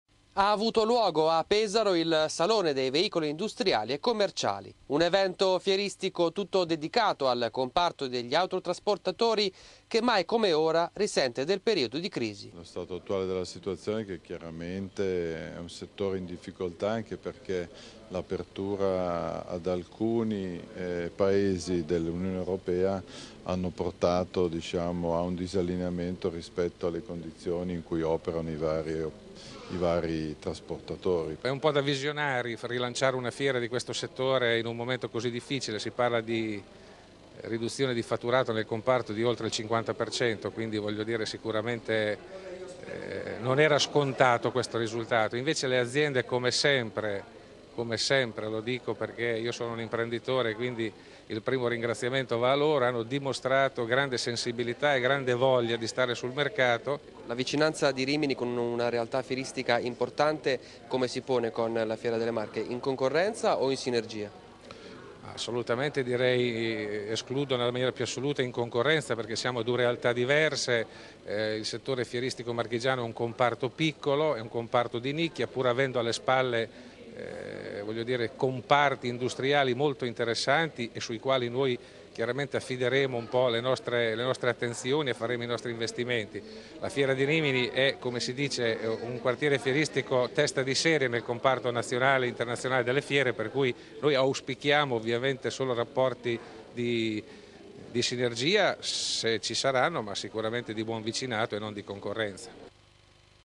Fiera di Pesaro: in esposizione veicoli industriali e commerciali
A Pesaro il Salone Veicoli Industriali e Commerciali.